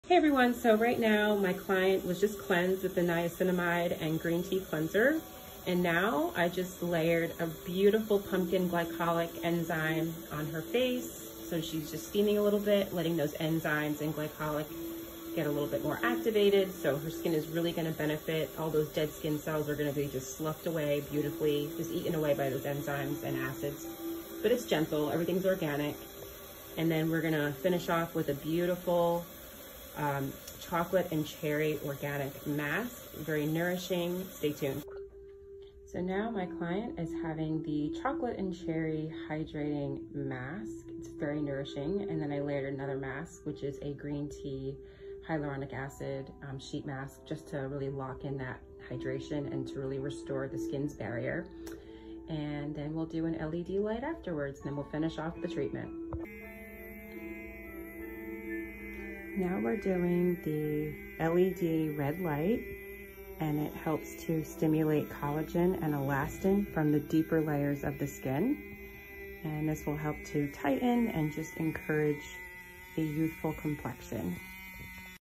My Holistic Facial 💆🏻‍♀️
My client’s in absolute bliss while her skin is getting it’s glow on with a holistic facial with zero harsh ingredients 🌱